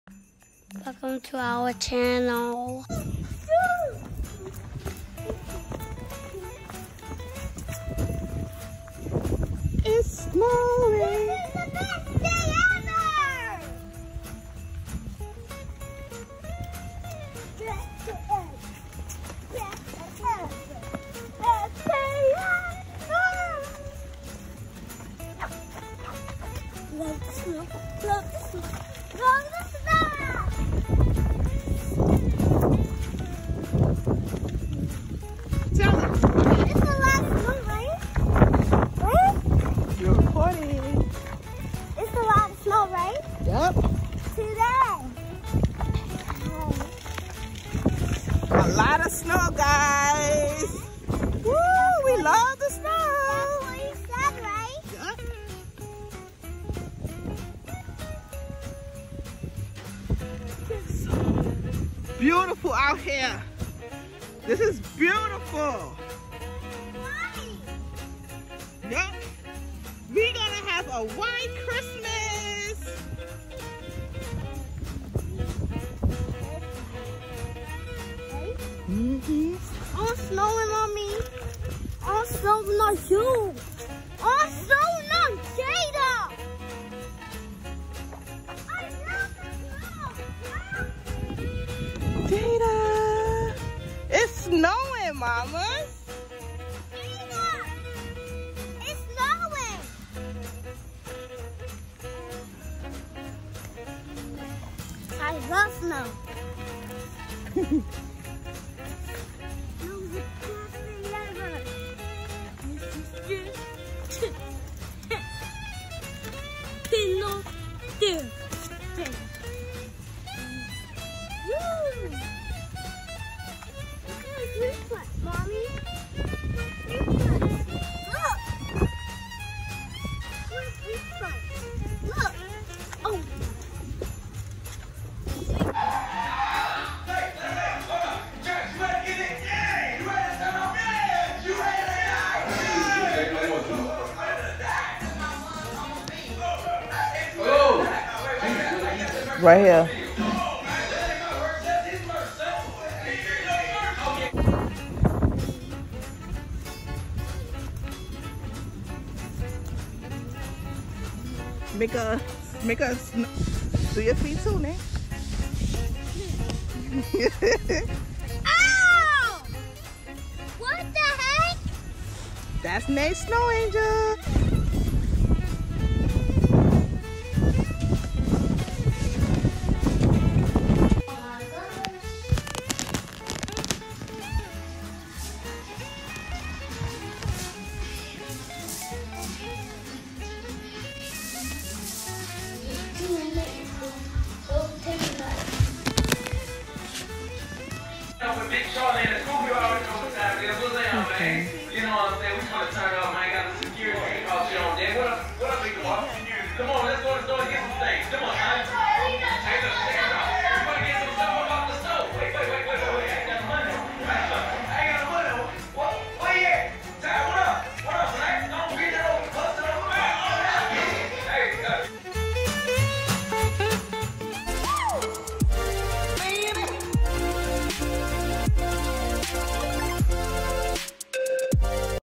SNOWWW!!! ITS SNOWING!!! THE BEST DAY EVER
IN THIS VIDEO YOU WILL SEE JANAE PLAYING OUTSIDE IN THE SNOW ON HER WAY HOME FROM SCHOOL